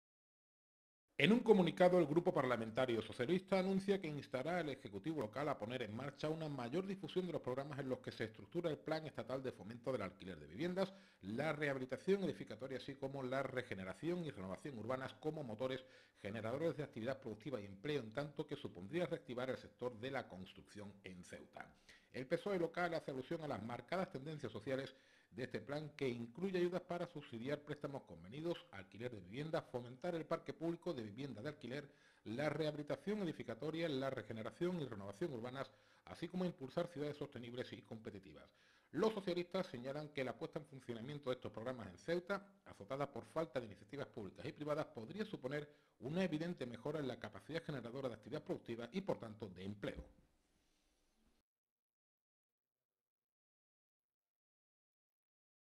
El PSOE exige al Gobierno que ponga en marcha el Plan Estatal de Vivienda
El Partido Socialista Obrero Español instará al Gobierno de la Ciudad a poner en marcha el Plan de vivienda 2013, con el objetivo de contribuir a la reactivación económica de Ceuta